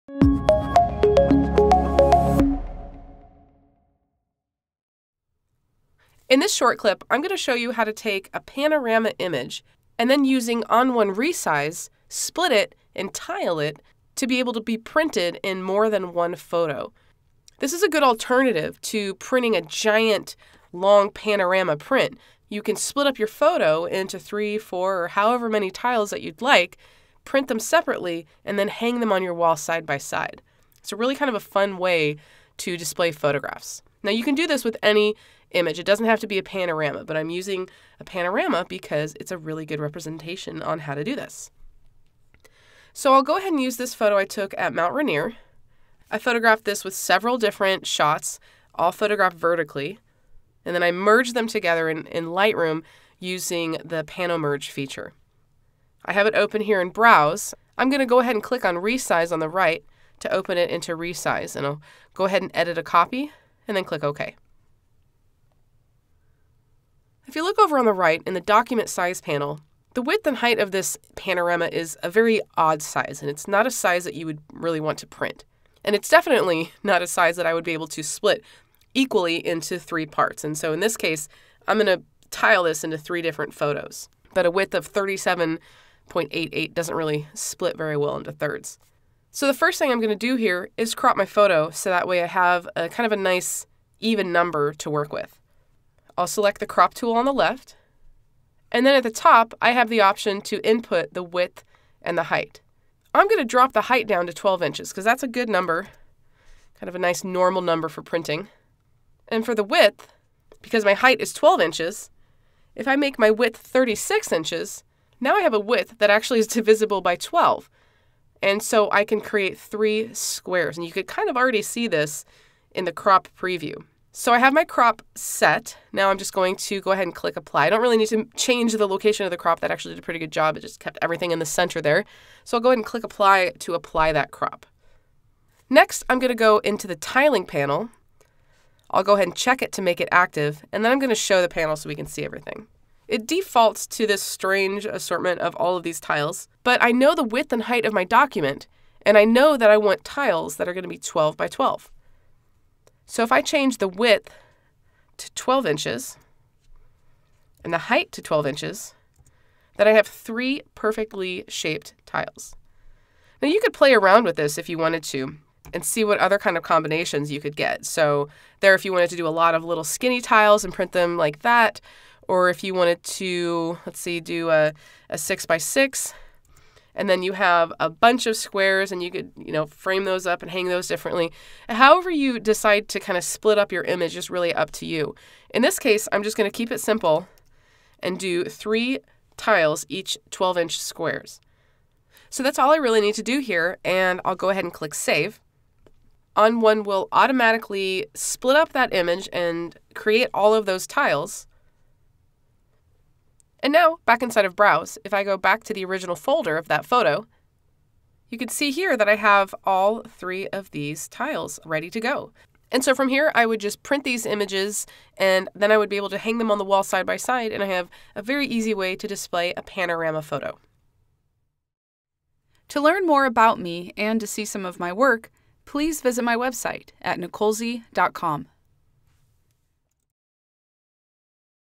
ON1 Short Clip - How to Print a Panorama on Multiple Pages
In this short clip, Nicole S. Young demonstrates how to take a panorama and tile it into three different pages using ON1 Resize.